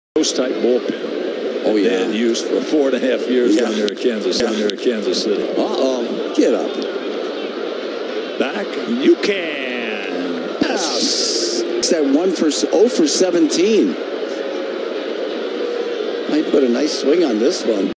Yolmer Sanchez a solo homer
Yolmer Sanchez gets the White Sox on the board in the 4th inning with a solo homer to right field